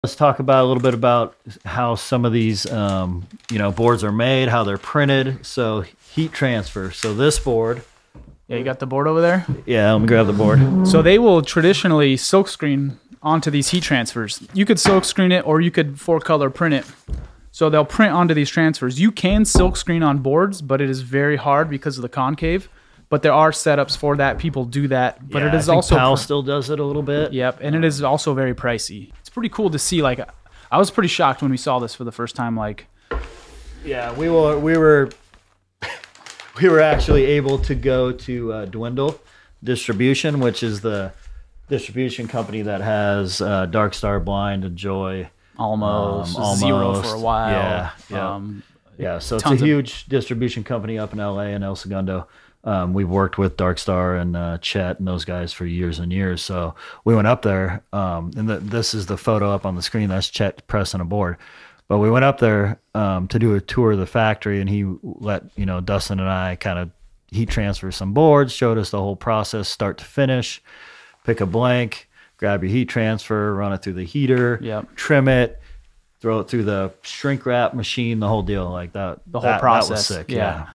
Lincoln Cuts - How graphics are applied to skateboards
We were blown away when we first saw the process of transferring artwork to a deck. In this cut from Lincoln Design Co's Deep Dive into Skateboard Graphics, Dan and Dustin walk you through the heat transfer process used by a number of big skate brands.

Visit the Lincoln Design Co shop!

Follow Lincoln Design Co. on Instagram